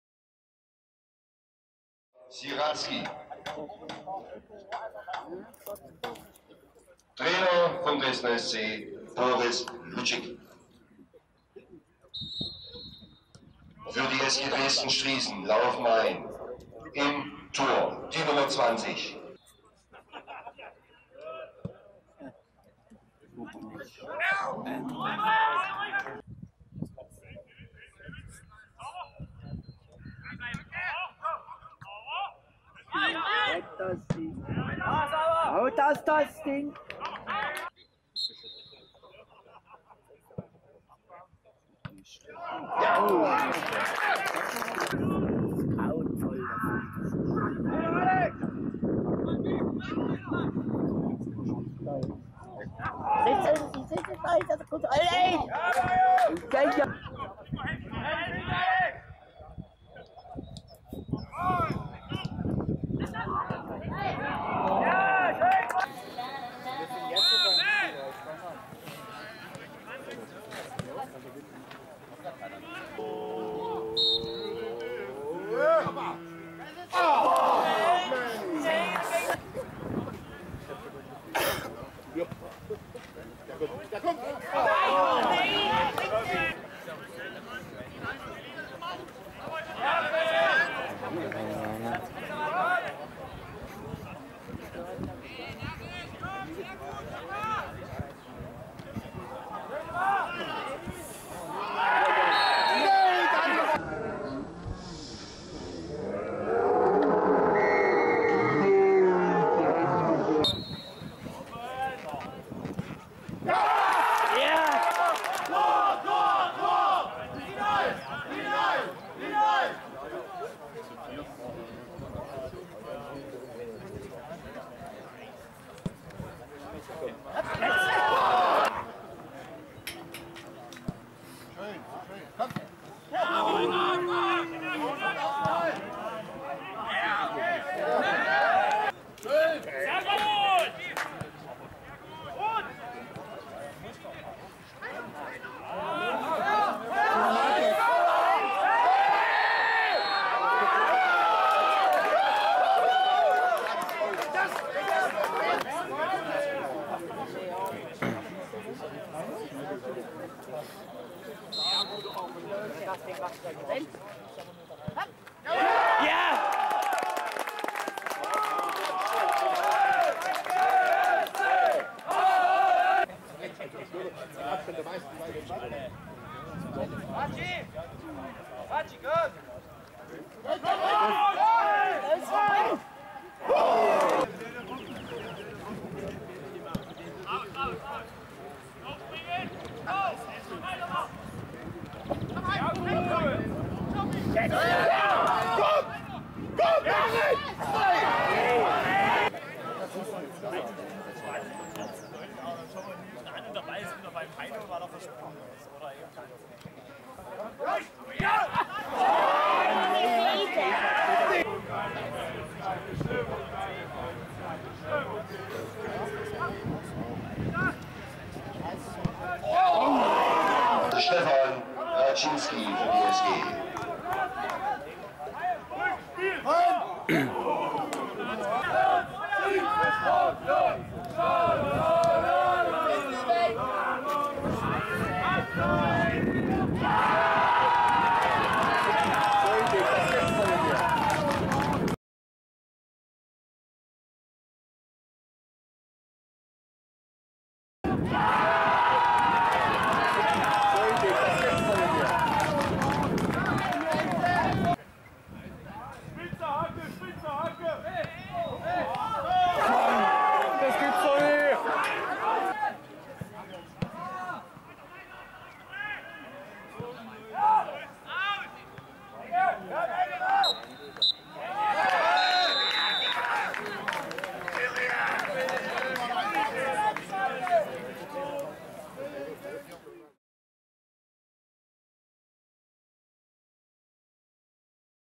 Highlights: SG Dresden Striesen - Dresdner SC 1898
Die Zusammenfassung der Partie zwischen der SG Dresden Striesen und dem Dresdner Sportclub 1898 vom 16. Spieltag der Stadtoberliga Dresden.